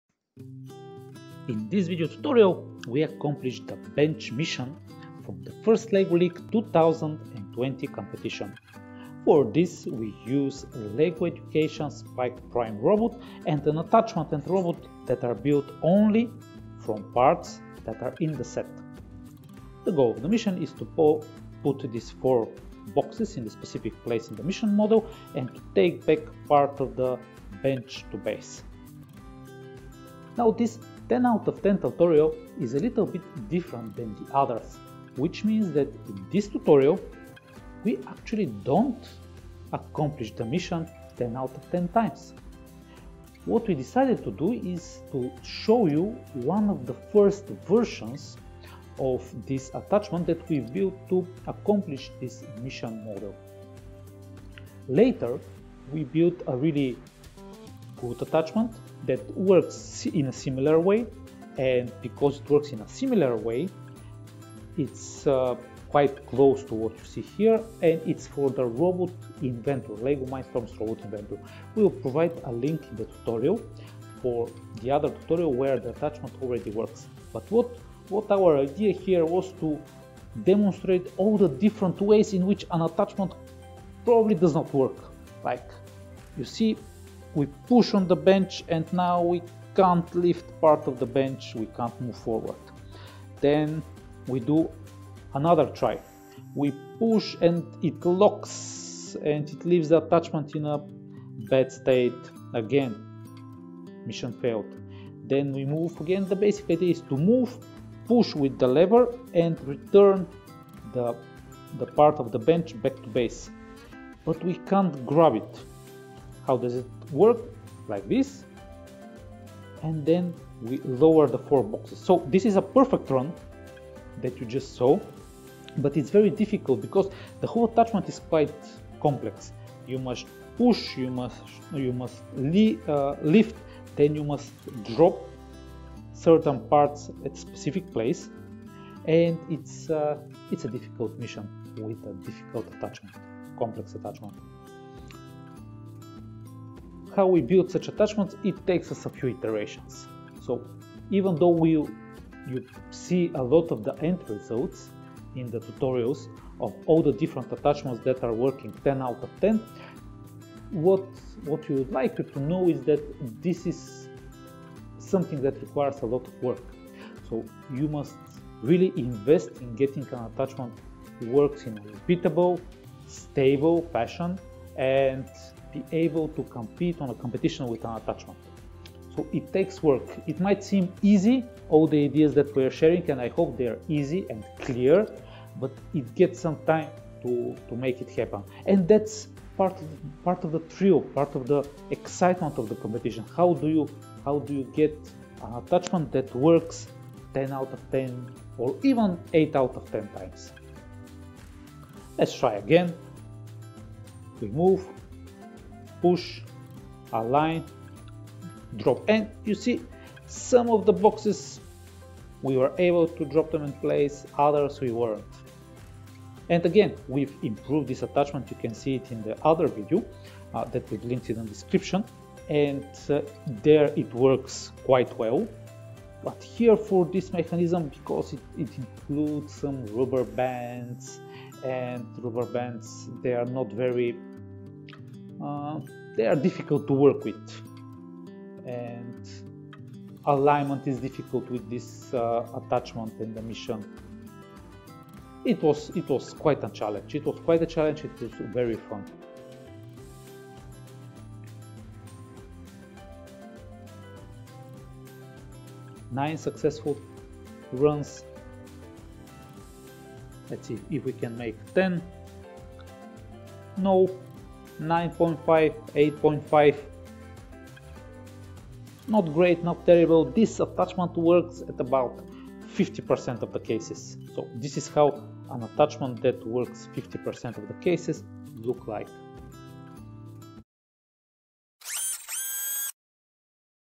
FLL 2020: 5 out of 10 on how to accomplish the Bench mission (FREE)
This is a 5 out of 10 video tutorial we take a different approach - we demonstrate how difficult it is to accomplish a mission. We are accomplishing the FIRST LEGO League 2020 Bench Mission with a LEGO Education SPIKE Prime robot. The attachment works 5 out of 10 times. Our goal with the video is to give everybody the understanding that taking your time to accomplish a mission is the right thing.

We would be glad if you share your thoughts with us in the comments below 👇 If you liked this video, don't forget to hit the 👍